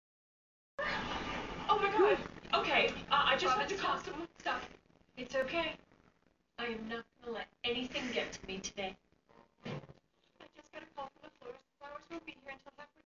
Blaisey Video
Blaisey the bengal kitty wakes up